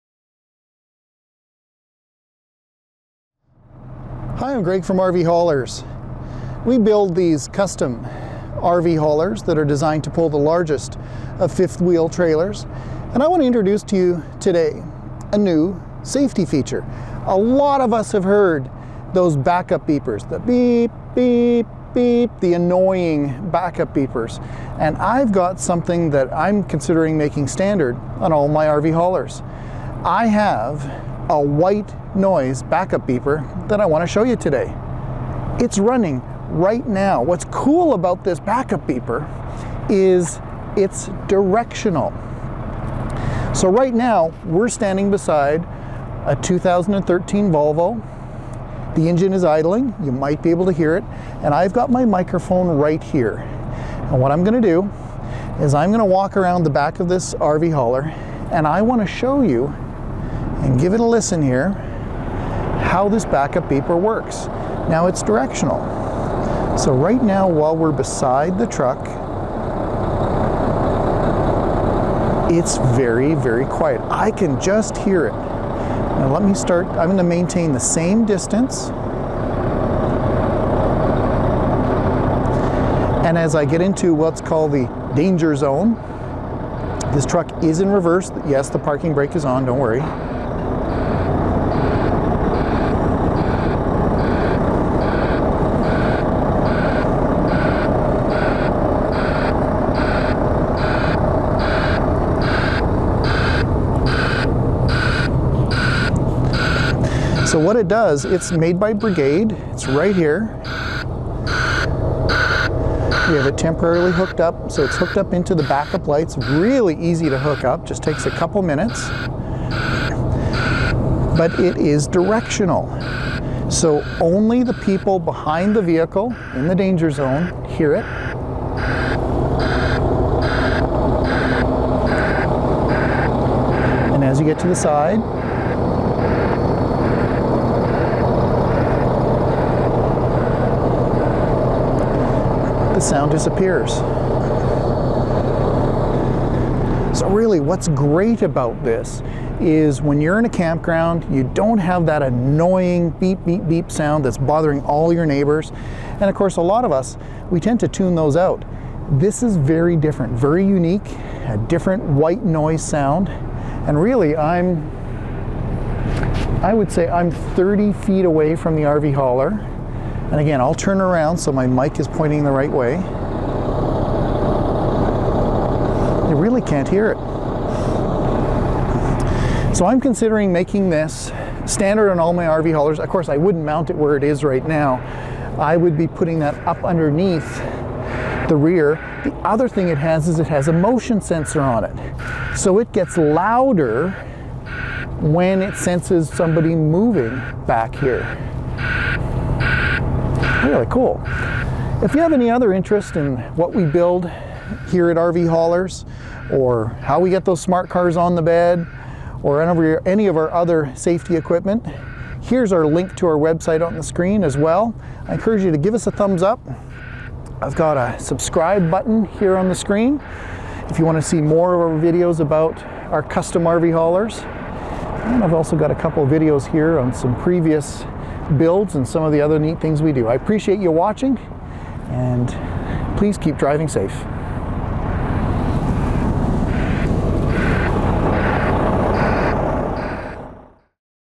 The Coolest Backup Beeper on an RVHauler White Noise Backup
Gregg from Rvhaulers.ca demonstrates a white-noise backup alarm.  It is effective, and not annoying to your campground neighbors.